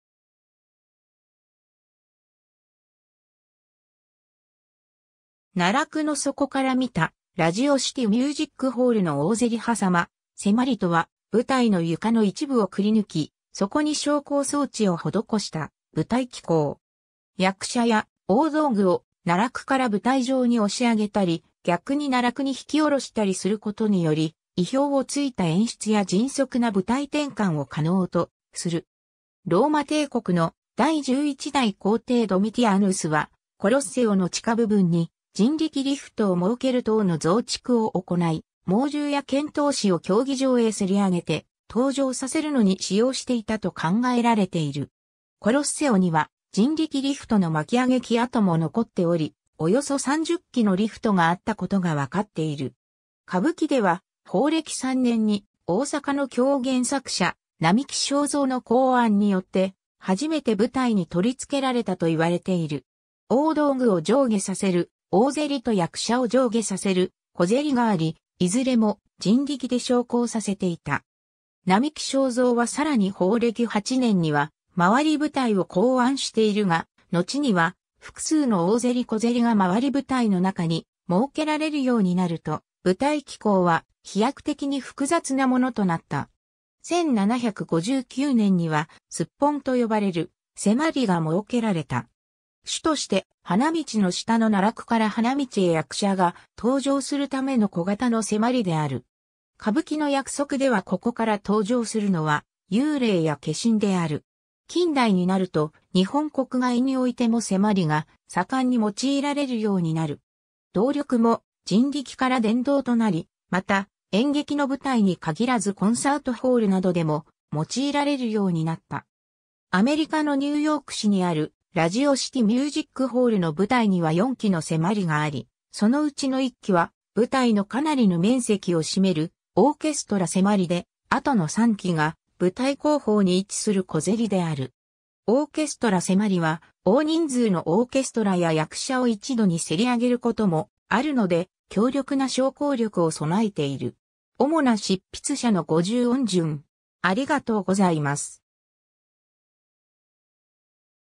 迫
奈落の底から見たラジオシティ・ミュージックホールの大迫り 迫・迫り（せり）とは、舞台の床の一部をくりぬき、そこに昇降装置を施した舞台機構。
役者や大道具を奈落から舞台上に押し上げたり（迫り上げ）、逆に奈落に引き下ろしたり（迫り下げ）することにより、意表をついた演出や迅速な舞台転換を可能とする。
 ローマ帝国の第11代皇帝ドミティアヌスは、コロッセオの地下部分に人力リフトを設ける等の増築を行い、猛獣や剣闘士を競技場へ迫り上げて登場させるのに使用していたと考えられている。
コロッセオには人力リフトの巻き上げ機跡も残っており、およそ30基のリフトがあったことが解っている。
 歌舞伎では、宝暦3年（1753年）に大坂の狂言作者・並木正三の考案によってはじめて舞台に取り付けられたといわれている。
大道具を上下させる「大迫り（おおぜり）」と役者を上下させる「小迫り（こぜり）」があり、いずれも人力で昇降させていた。
並木正三はさらに宝暦8年には廻り舞台を考案しているが、のちには複数の大迫り・小迫りが廻り舞台の中に設けられるようになると、舞台機構は飛躍的に複雑なものとなった。
 1759年にはすっぽんと呼ばれる迫りが設けられた。
主として、花道の下の奈落から花道へ役者が登場するための小型の迫りである。
歌舞伎の約束ではここから登場するのは、幽霊や化身（狐が化けた人間など）である。
 近代になると日本国外においても迫りが盛んに用いられるようになる。
動力も人力から電動となり、また演劇の舞台に限らずコンサートホールなどでも用いられるようになった。
アメリカのニューヨーク市にあるラジオシティ・ミュージックホールの舞台には4基の迫りがあり、そのうちの1基は舞台のかなりの面積を占める「オーケストラ迫り」で、あとの3基が舞台後方に位置する小迫りである。
オーケストラ迫りは大人数のオーケストラや役者を一度に迫り上げることもあるので、強力な昇降力を備えている。
 主な執筆者の50音順。